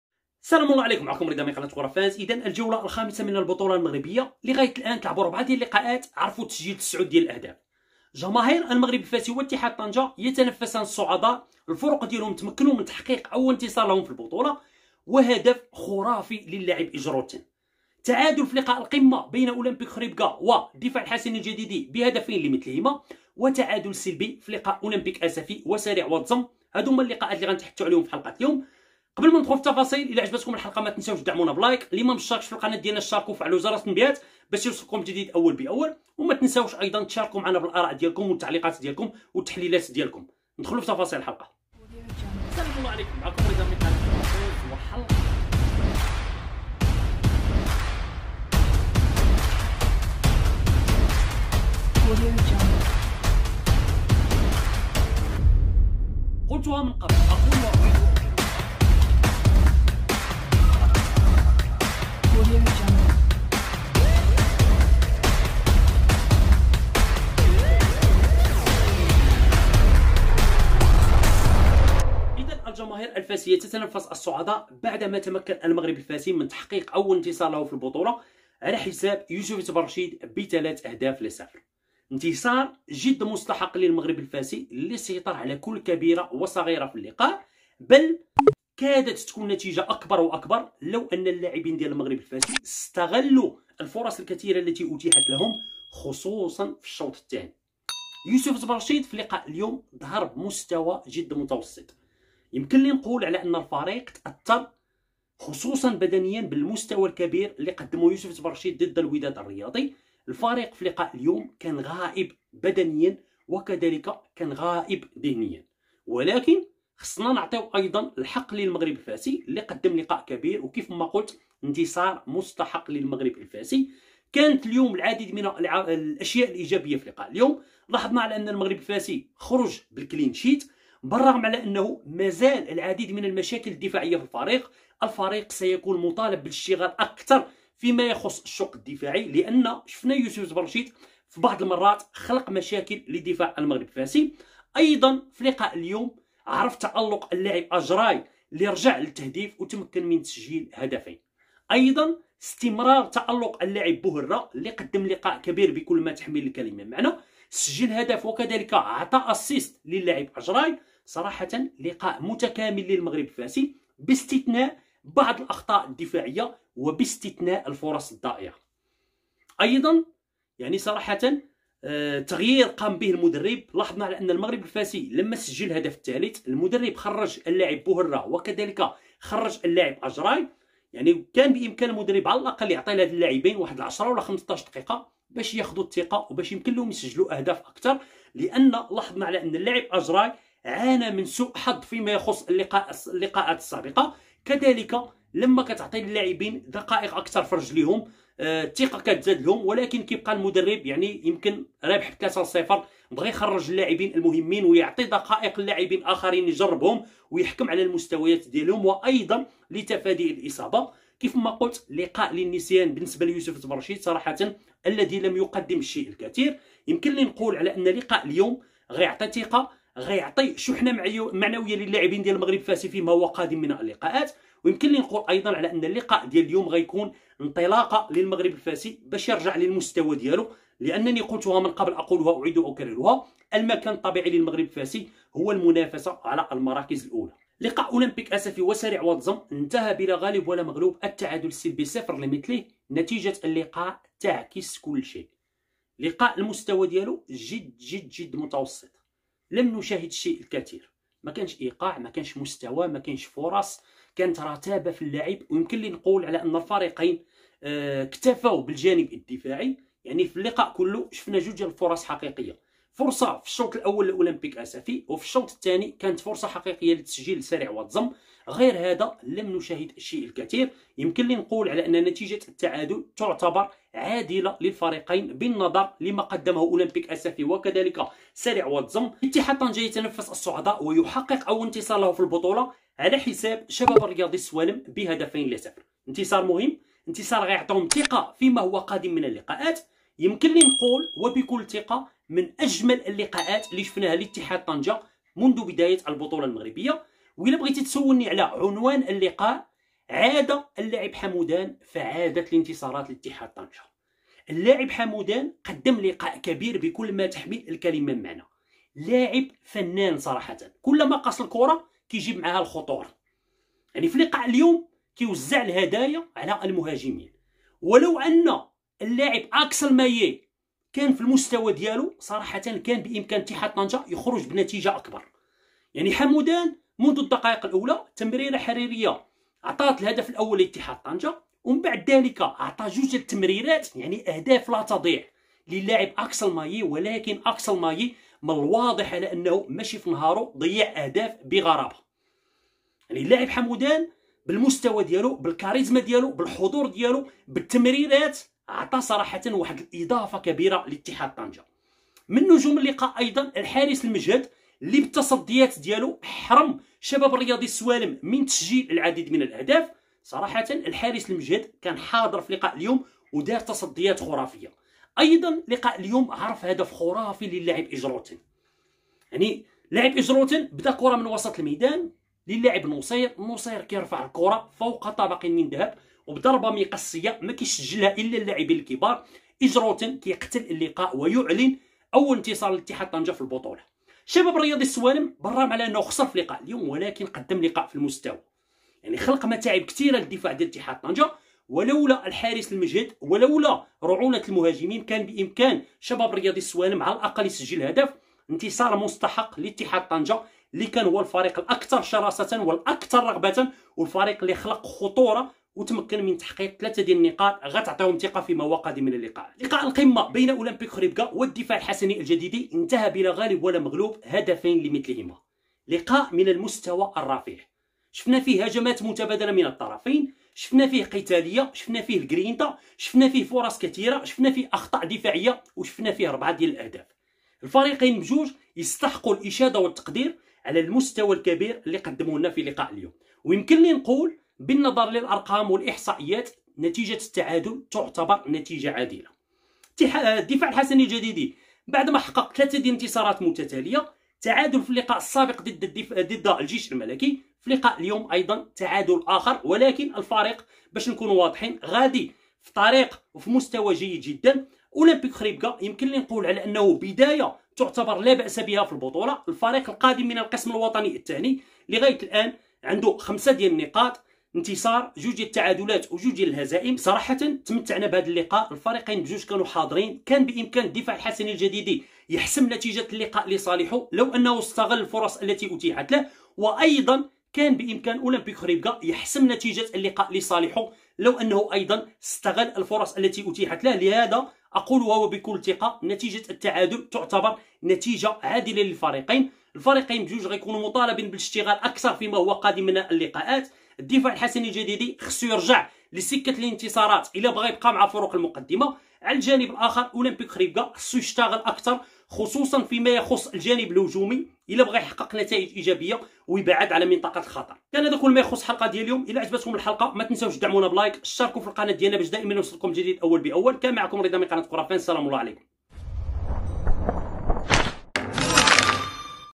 أجراي يقود الماص لاكتساح ي.برشيد بثلاثية/عاد حمودان فعادت اتحاد طنجة/وتعادل بين أ.خريبكة والدفاع..
الجولة _الخامسة _من_البطولة_المغربية#
المغرب الفاسي يوسفية برشيد 3~0
اولمبيك اسفي سريع واد زم 0~0
شباب السوالم اتحاد طنجة 0~2
اولمبيك خريبكة الدفاع الحسني الجديدي 2~2